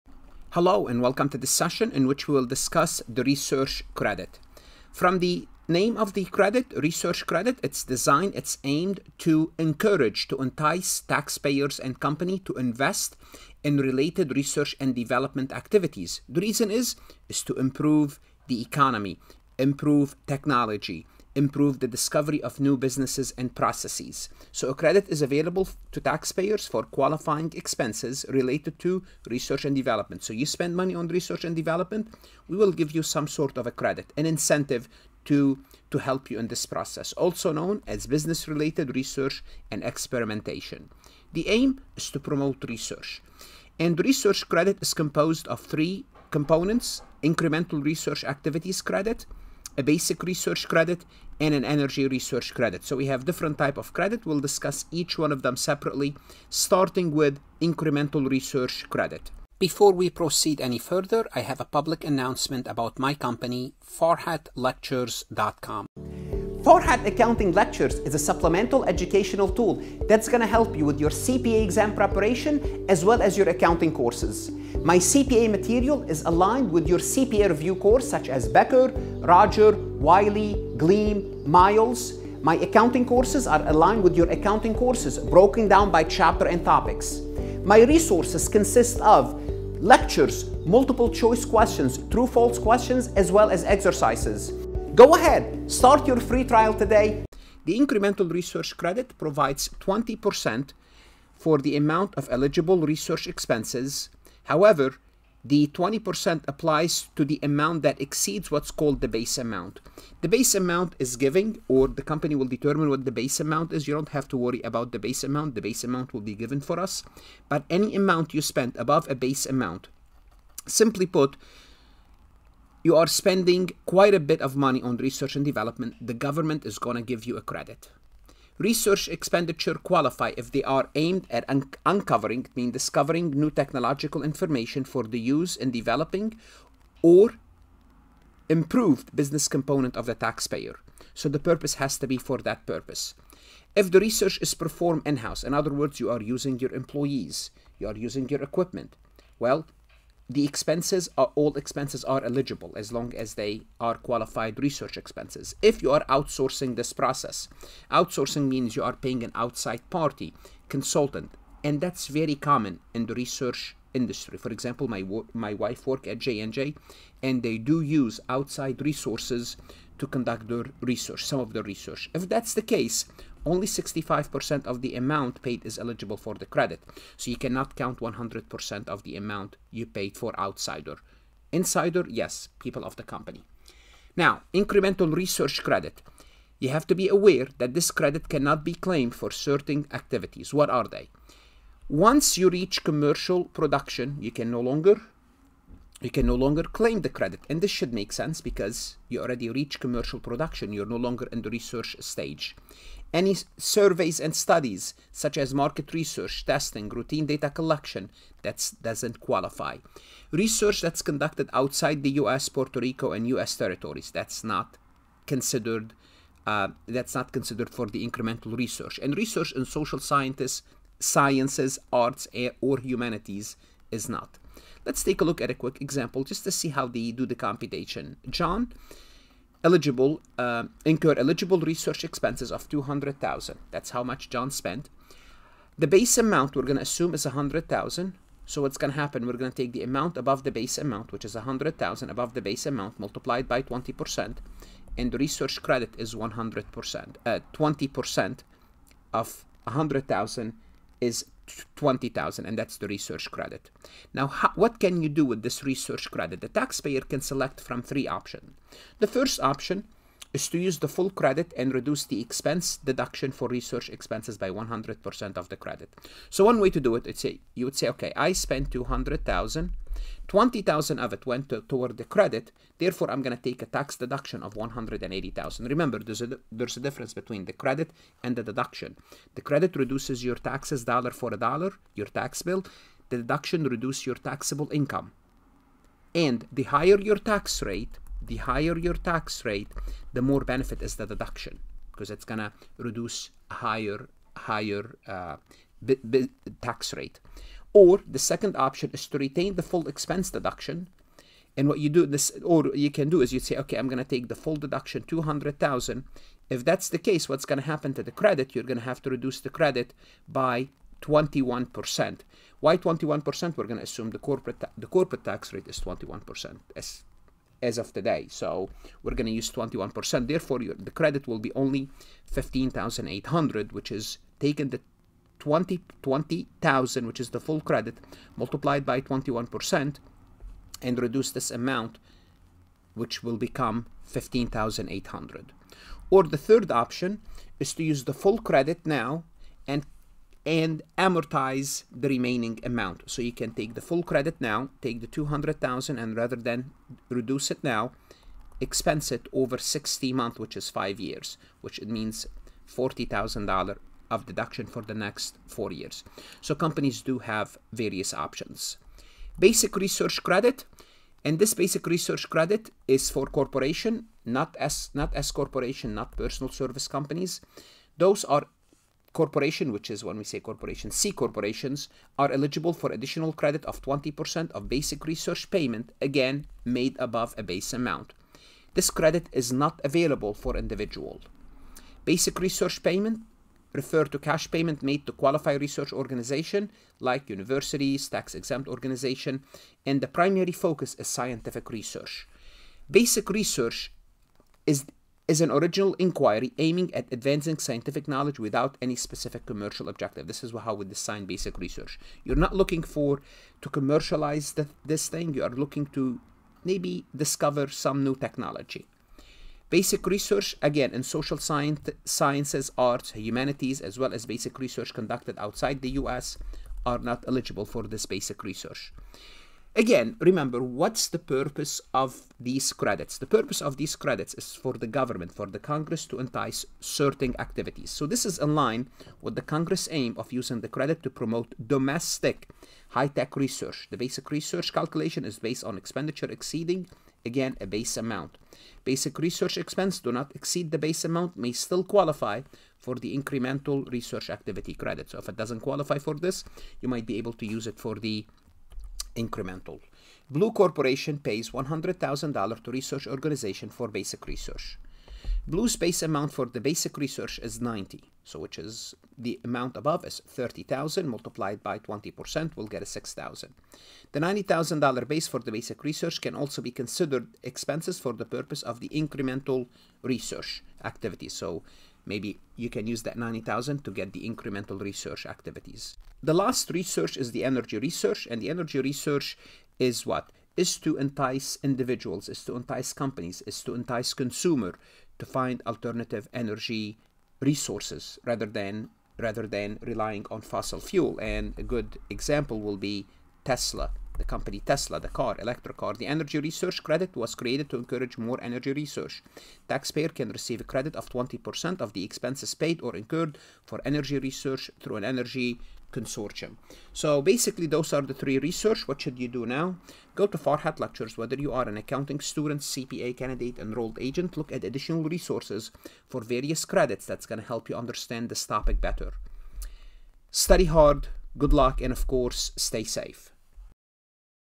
Tax Research Credits. CPA Exam
In this session, I discuss the tax research credits